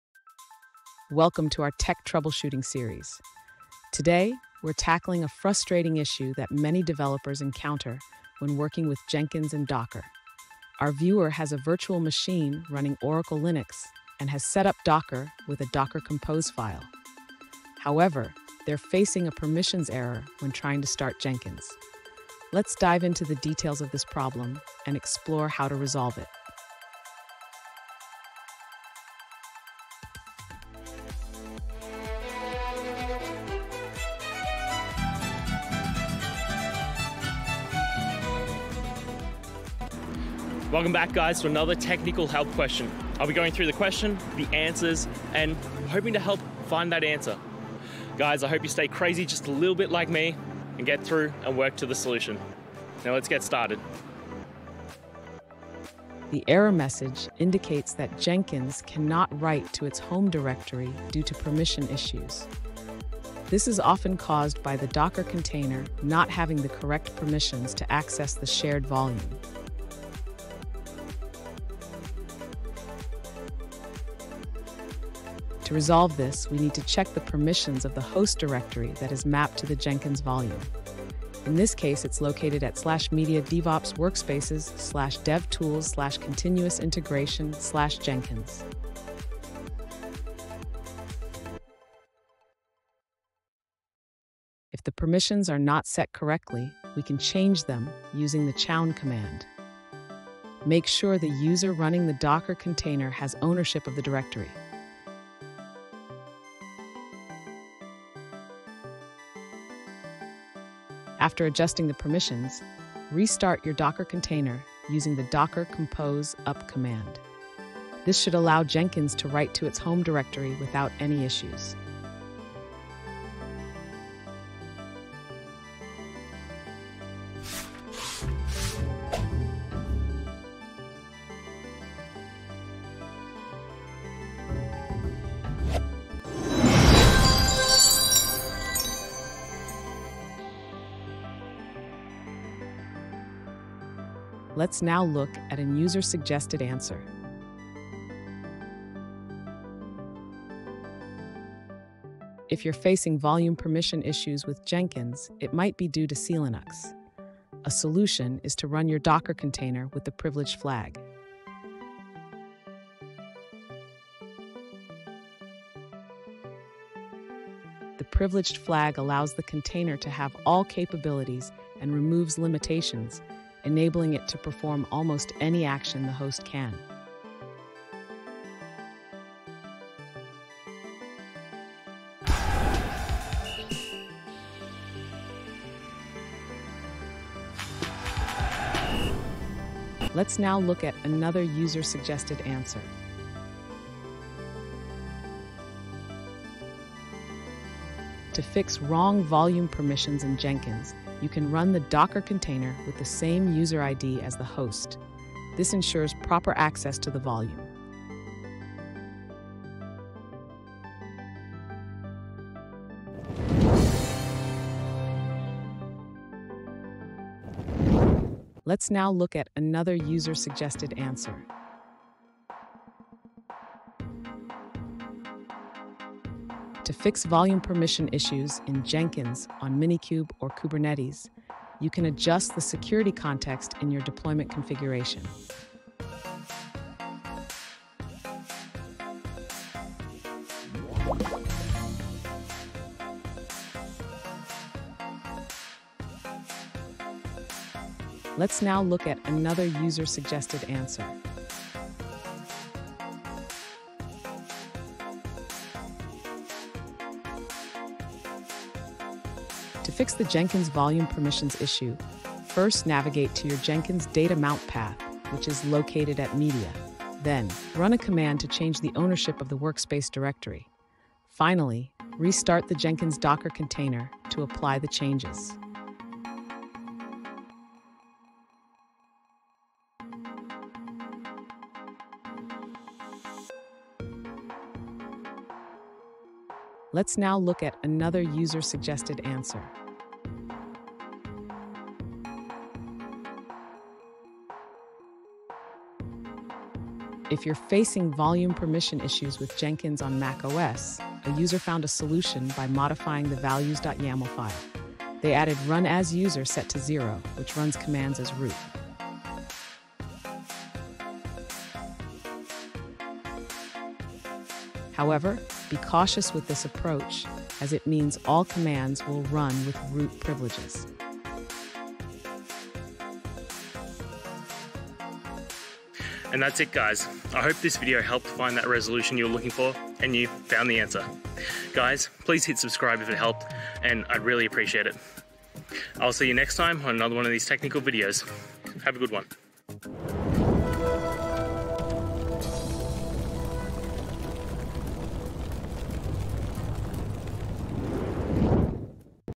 Fixing Jenkins Wrong Volume Permissions: Troubleshooting Guide & Solutions
In this video, we dive into a common issue faced by Jenkins users: incorrect volume permissions. These permission problems can lead to frustrating build failures and hinder your CI/CD pipeline. Join us as we explore effective troubleshooting techniques and practical solutions to ensure your Jenkins environment runs smoothly and efficiently. Whether you're a seasoned developer or just starting out, this guide will help you tackle volume permission issues with confidence.

Today's Topic: Fixing Jenkins Wrong Volume Permissions: Troubleshooting Guide & Solutions

Related to: jenkins, volumepermissions, fixingjenkins, troubleshootingguide, jenkinssolutions, jenkinserrors, volumeissues, permissionerrors, ci/cd,